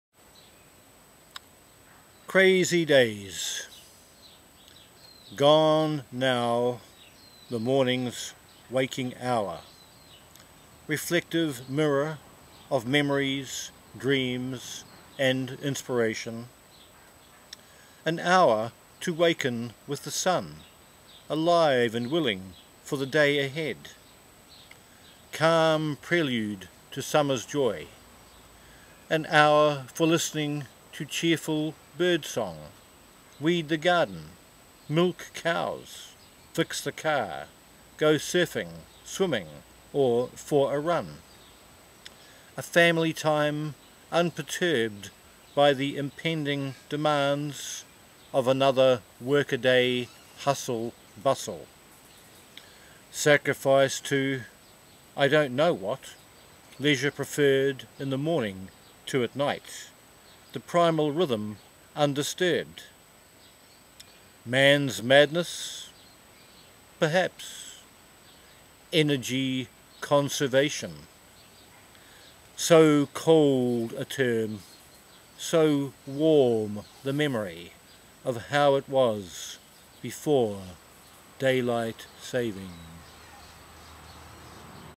Roark Watson Poetry Live - 'Crazy Dayze' take 1
At last folks,
A modest selection of 10 poetry readings, recorded in the 'Backyard Studio' - basically me and Mac Book, out the rear door of my South Pacific epicentre, Te Awanga, New Zealand - hopefully for your enjoyment, derision or laughter! Lots more in reserve, but a surprising effort getting even these simple items' up. Experiencing your own work via this medium is most helpful, with improvements savvied for every level of future productions. Pimply as conditions are, all comment very welcome.
Thanks again for your support and patience,
Roark